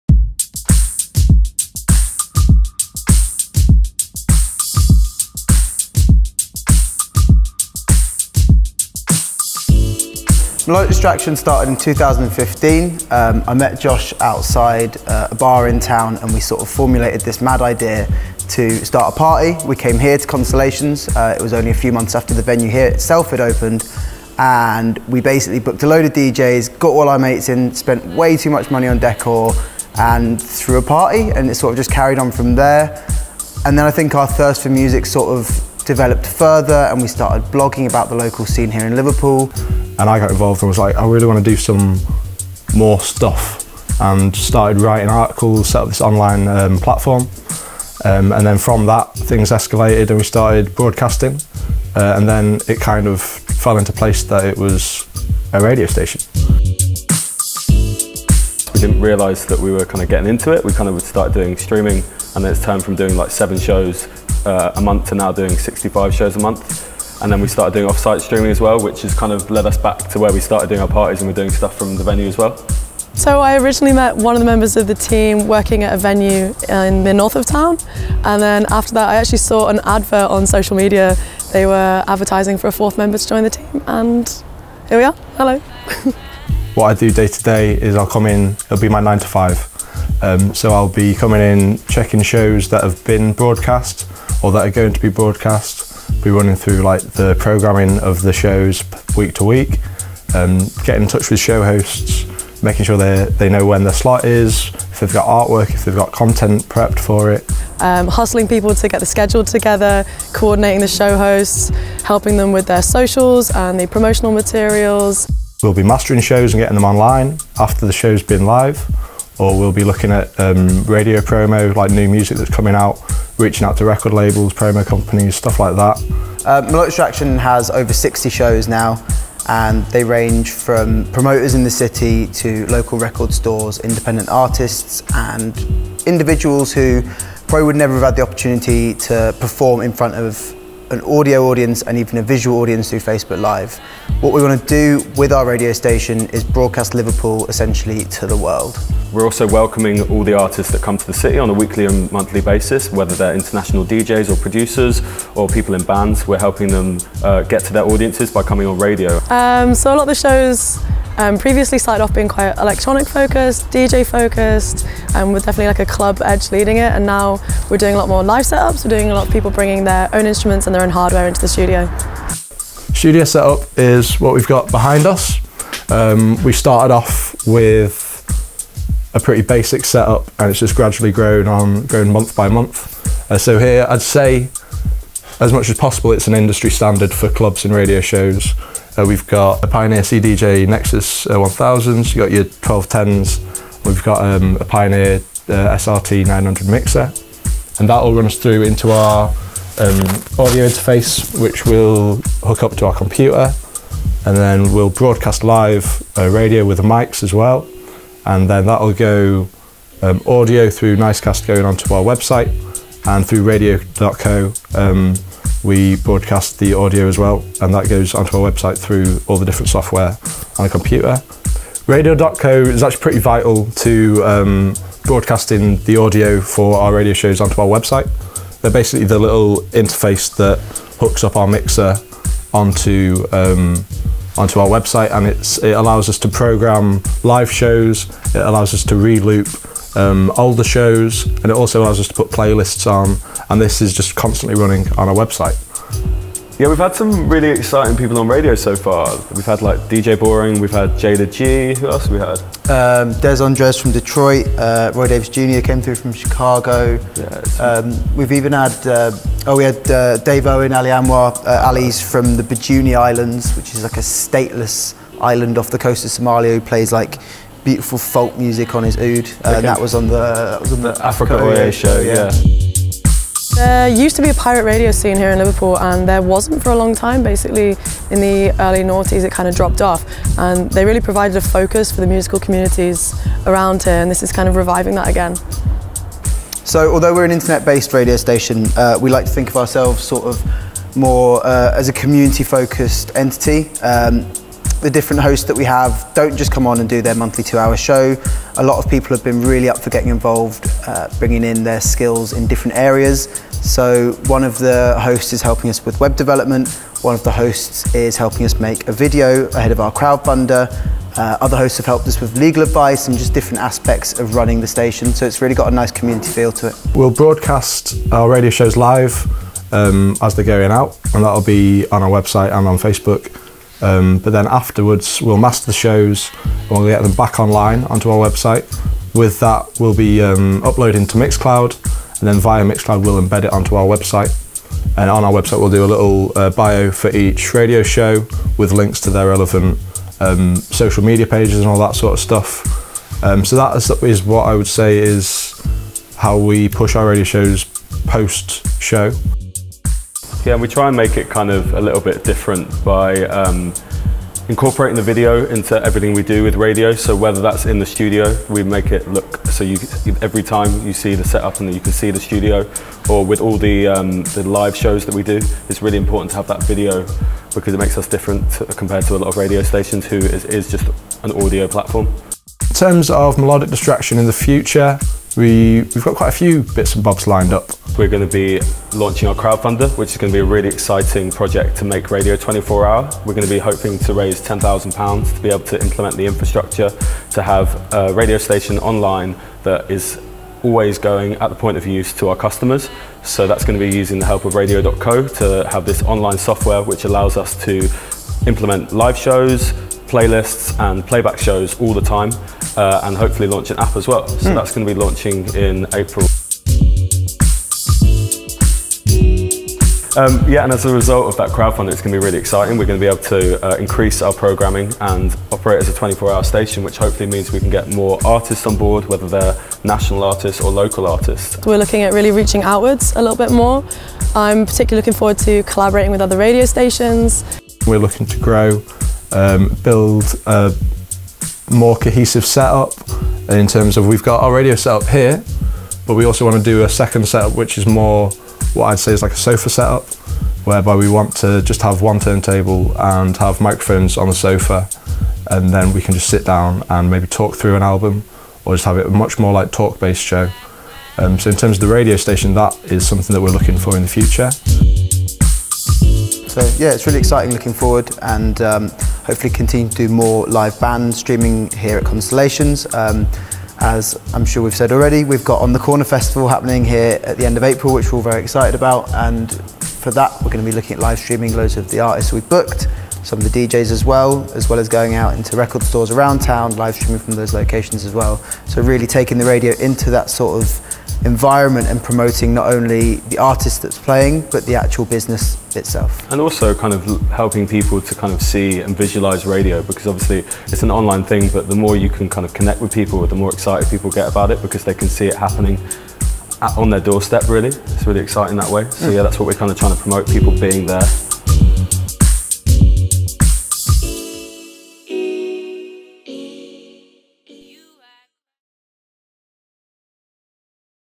Melodic Distraction on How to Live Stream a DJ Set
Melodic Distraction are an internet radio station, online magazine, and events programmer situated in the heart of Liverpool. Here's how they live stream DJ sets with the help of Radio.co.